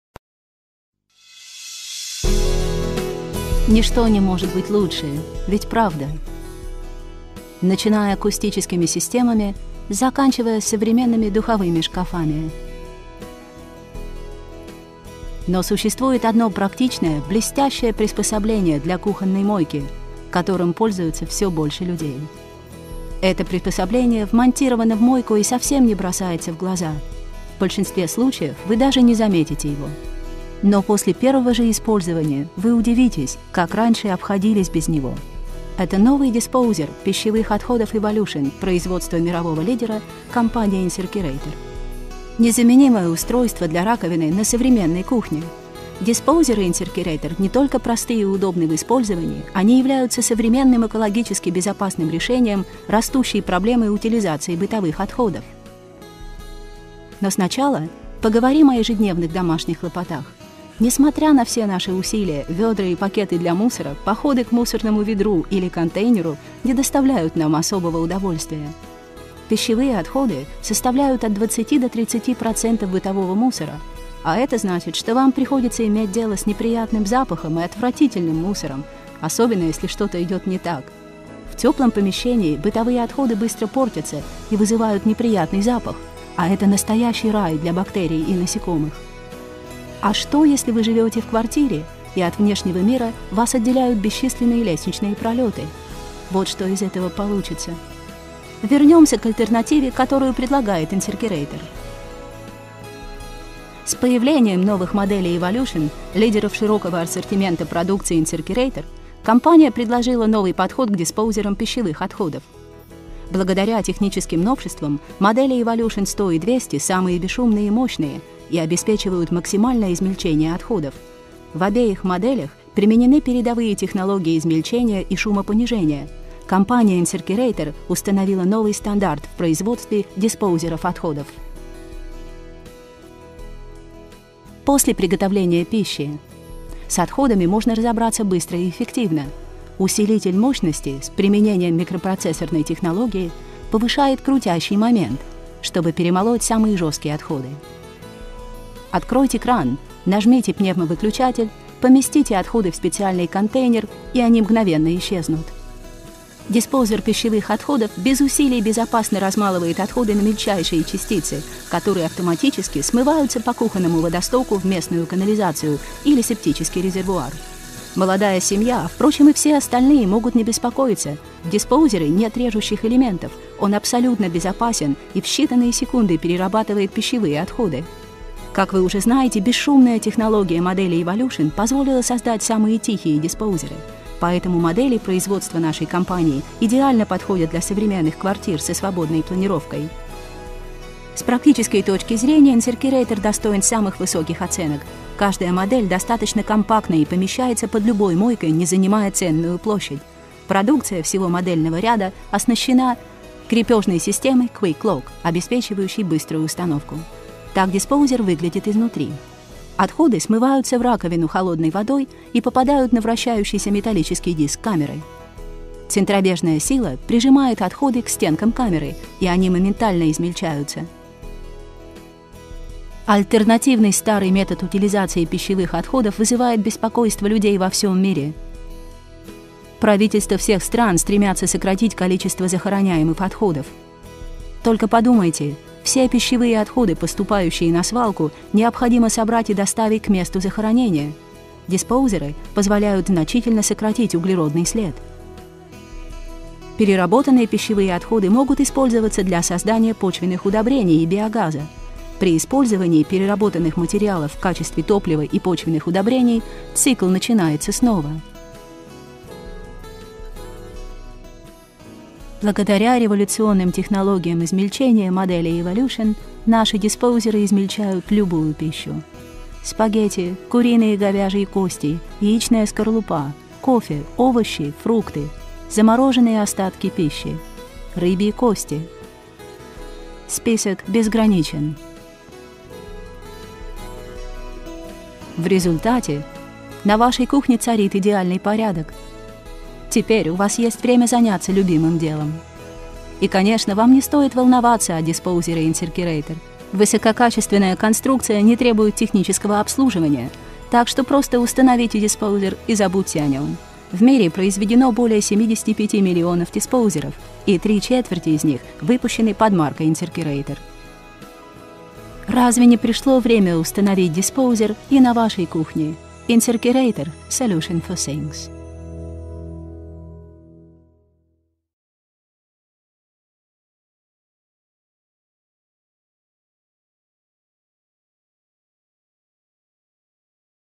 Измельчитель пищевых отходов IN SINK ERATOR ISE EVOLUTION 100
Напряжение питания: 220-240 В.;
Мощность измельчителя - 0.70 л.с.;
На 40% тише своих предшественников;
Автоматическая защита диспозера от перегрузки;
Двухуровневая система измельчения из нержавеющей стали;
Тип загрузки пищевых отходов - непрерывная загрузка;
Перемалывающая камера: объем - 1,005 л;
Авто-Реверс: позволяет двигателю при каждом включении вращаться в против